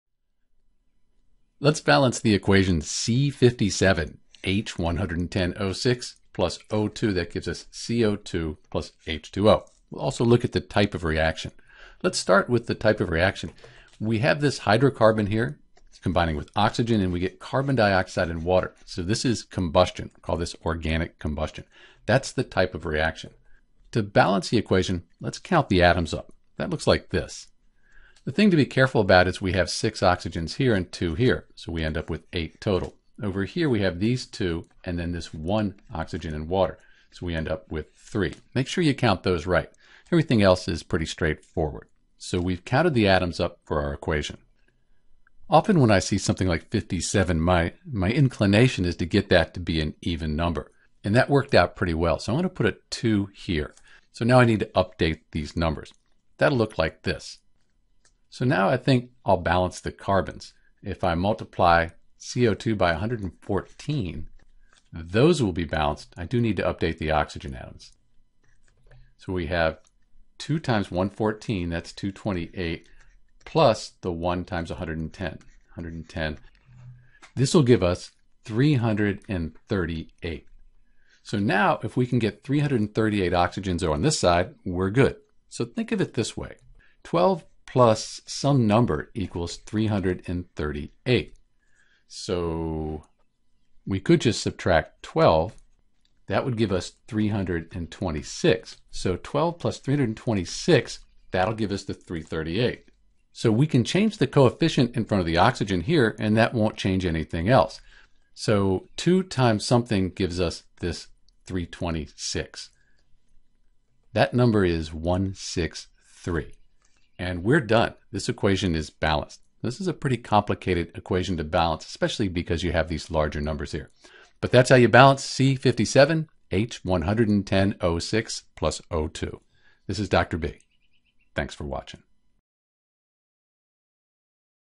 Balancing the Equation C57H110O6 + O2 = CO2 + H2O (and Type of Reaction)
To balance the chemical equation C57H110O6 + O2 = CO2 + H2O you first must correctly count all of atoms on each side of the chemical equation.

When we balance an equation like Combustion of Tristearin our goal is to provide the correct coefficients for each compound. The coefficient is the number that goes in front of the compound.

To balance C57H110O6 + O2 = CO2 + H2O you'll need to be sure to count all of atoms on each side of the chemical equation.

Once you know how many of each type of atom you can only change the coefficients (the numbers in front of atoms or compounds) to balance the equation for Combustion of Tristearin .